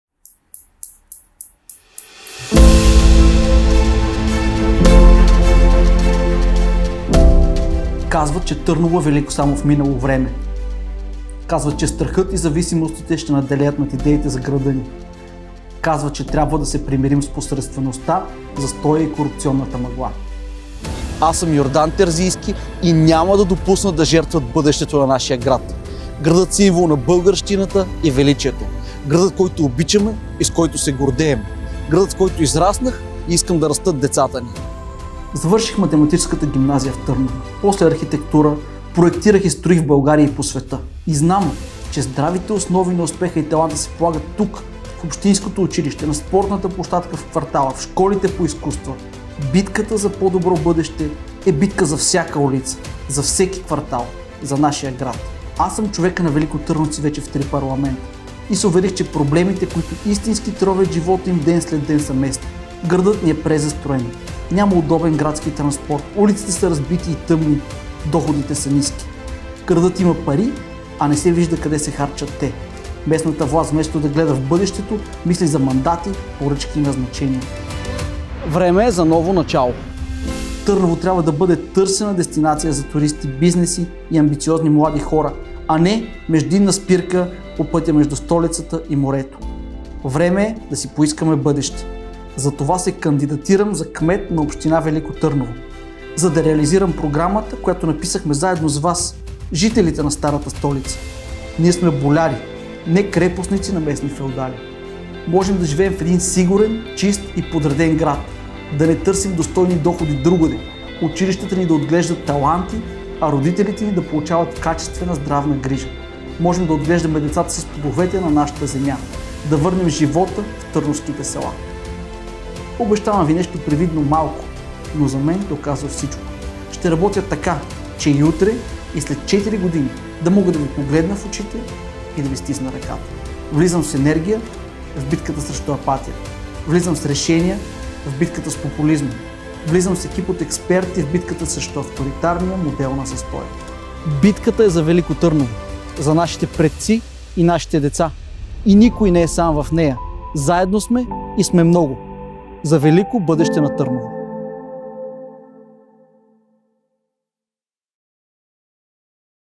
Казват, че Търново е велико само в минало време.
За велико бъдеще на Търново - Йордан Терзийски с бюлетина 66!
За България, в която корупцията намалява, а населението се увеличава
Последвайте ни!